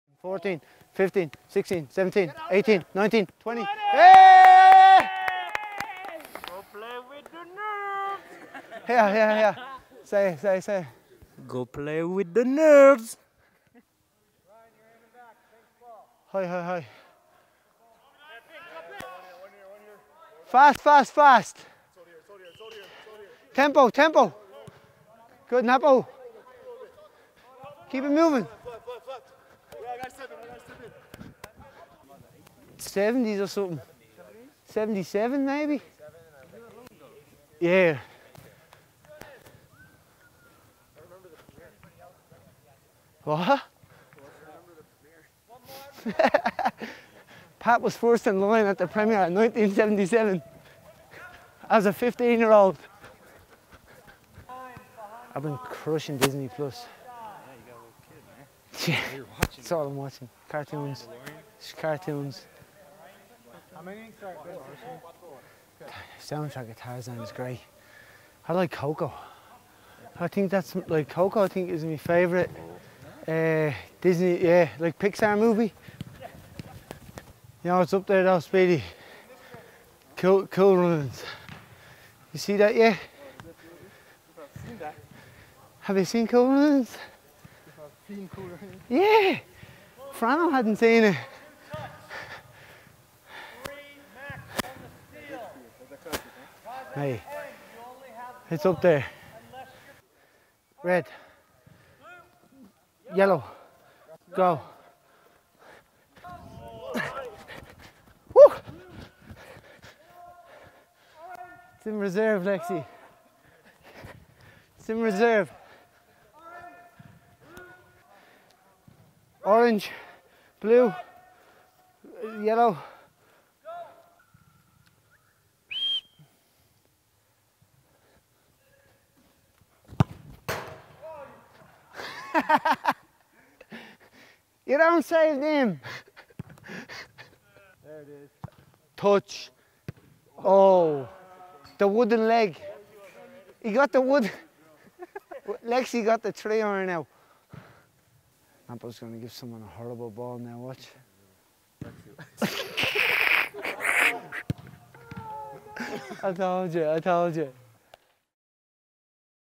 Niall McCabe Mic'd Up
During the Utah Trip, Niall McCabe was mic’d up during practice. Here it is, in all its glory!